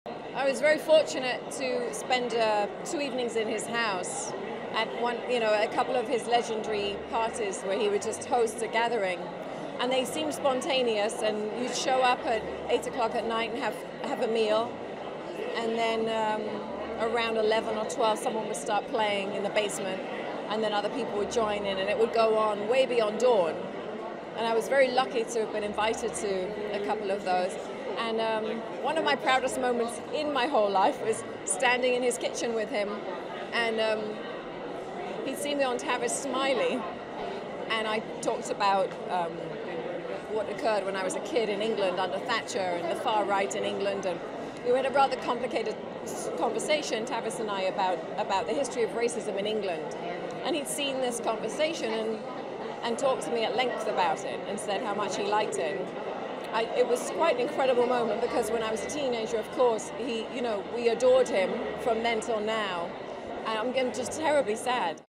Saffron Burrows' Conversation With Prince
At a special screening for 'Mozart in the Jungle,' star Saffron Burrows reminisces about her encounter with Prince and how their conversation became one of her 'proudest moments.' (April 22)